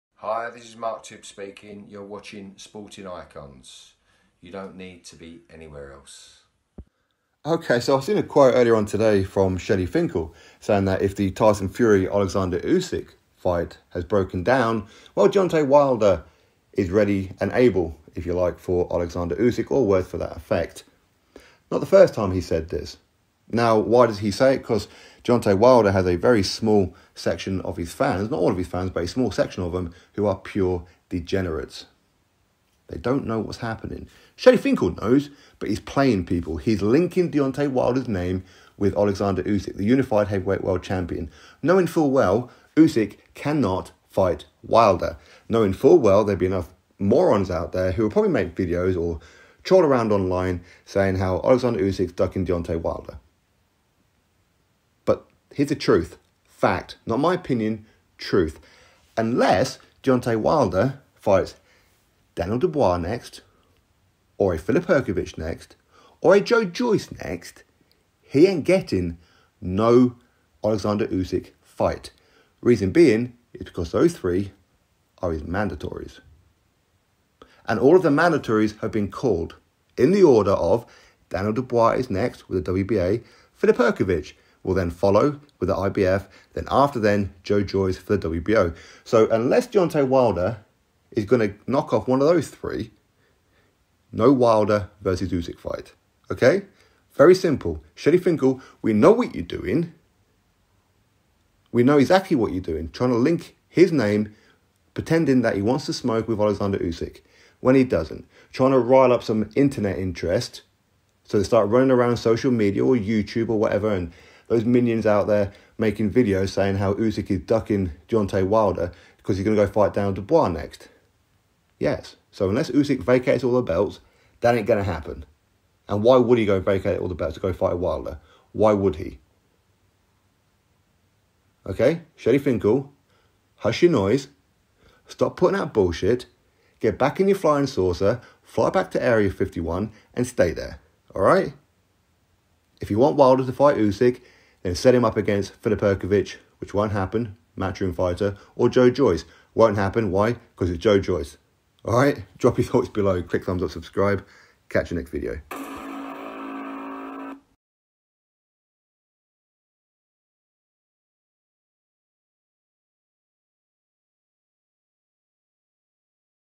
Deontay Wilder will take Oleksandr Usyk fight in a heartbeat says Team Wilder!!
Latest Boxing News: Deontay Wilder will take Oleksandr Usyk fight in a heartbeat says Team Wilder.